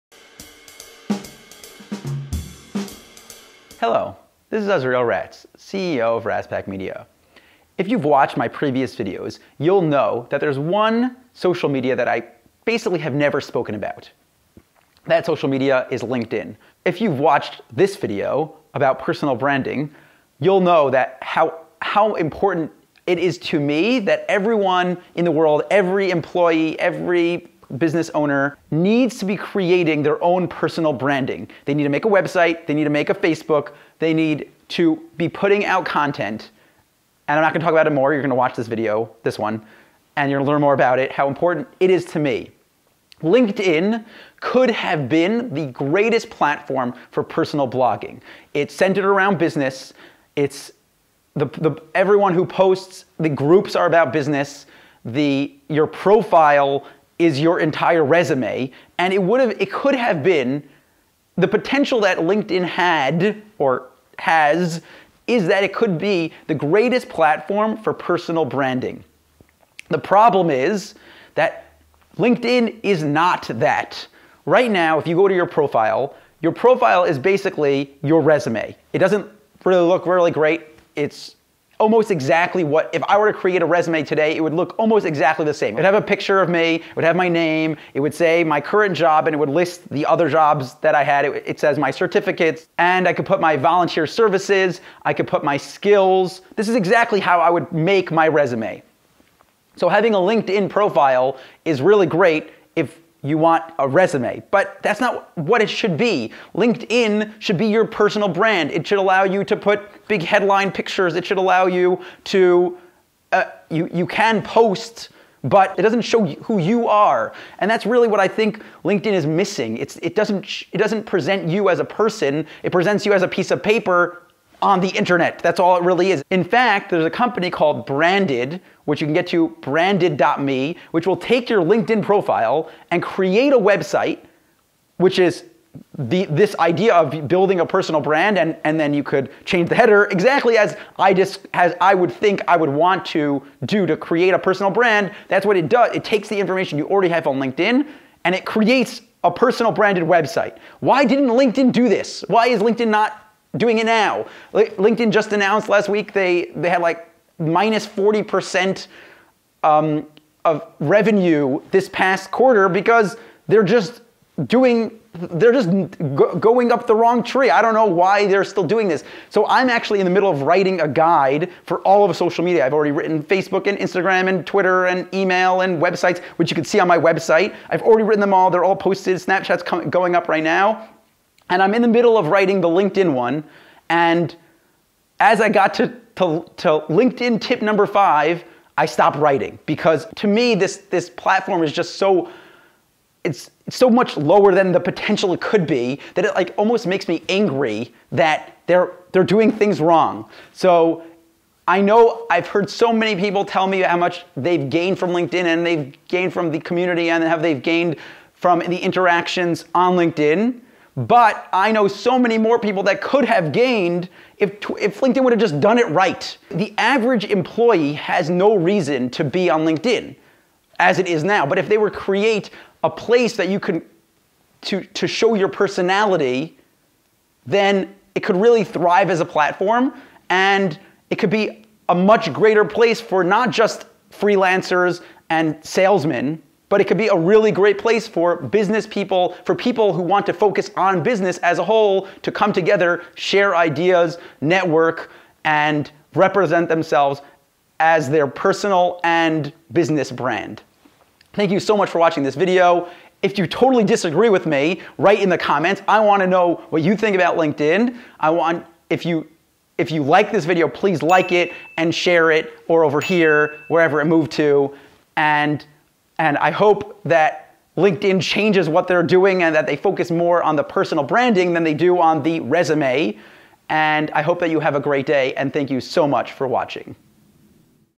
Why I Think LinkedIn Isn't Doing Well - Marketing Essentials
I think that LinkedIn could be an amazing platform for personal branding, but the platform is just doing things wrong. Here is what I think LinkedIn should do to be not just a place for salesmen, but to allow all people to create a place to build a personal brand.